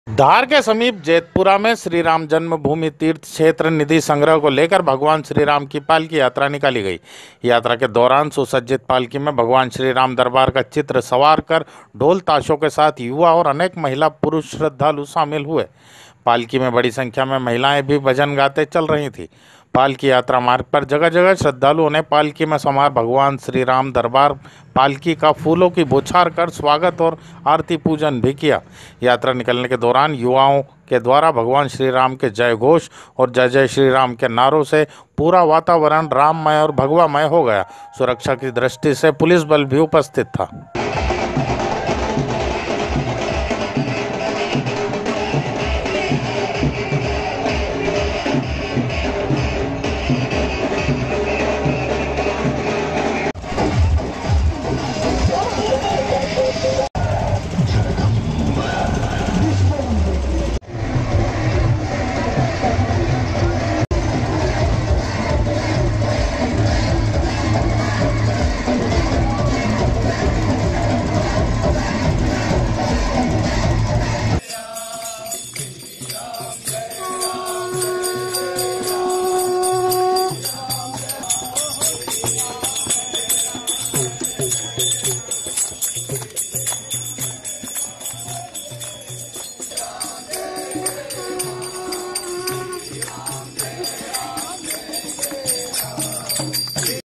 जेतपुरा में भी आज निधि संग्रहण अभियान के तहत निकली भव्य प्रभु श्रीराम पालकी यात्रा ग्रामीण उत्साहित
धार - प्रतिदिन प्रभात फेरी के साथ ही जेतपुरा में आज निधि संग्रहण अभियान के तहत निकली भव्य प्रभु श्रीराम पालकी यात्रा , पूरी खबर देखें लोकप्रिय केबल न्यूज़ धार न्यूज़ चैनल पर